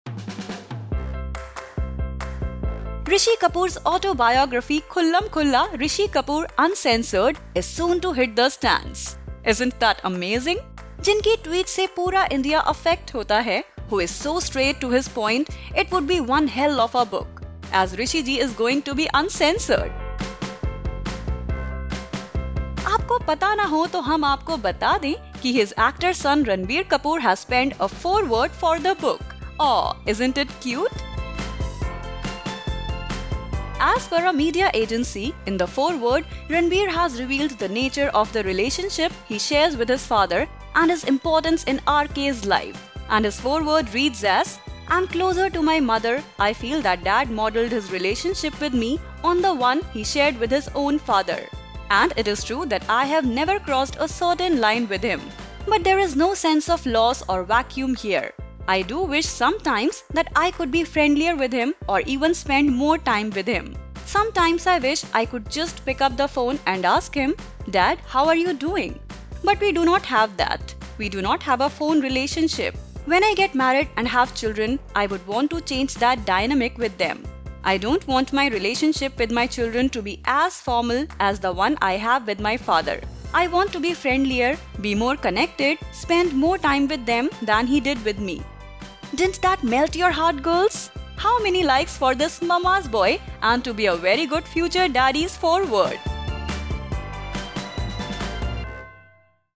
Ranbir Kapoor Explains his Relationship with Rishi Kapoor
Ranbir Kapoor is said to have written foreword for his dad’s upcoming autobiography where he spills the beans on his relations with dad, Rishi Kapoor, to know more watch the video.